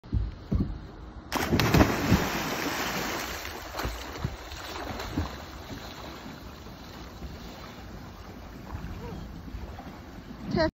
Early winter swim
Quick swim in cold water. Approx 8 degrees C.